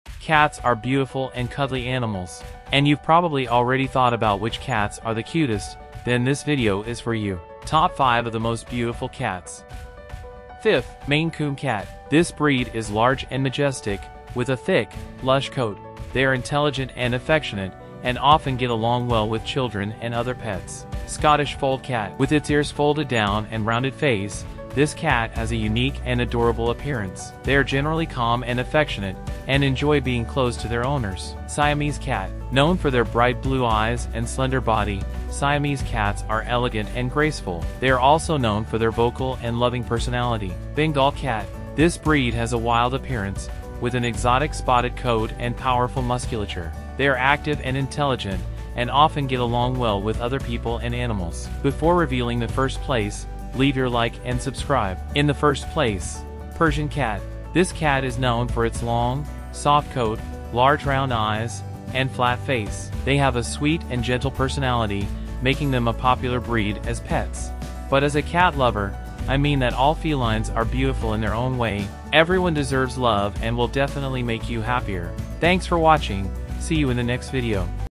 TOP 5 of the most beautiful cats
Are you a cat lover like me?
So at some point you must have thought, which ones are the most beautiful, this video will solve your doubt today!